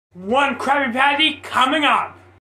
SpongeBob saying "One Krabby Patty, coming up!" but he's voiced by me
COMING UP!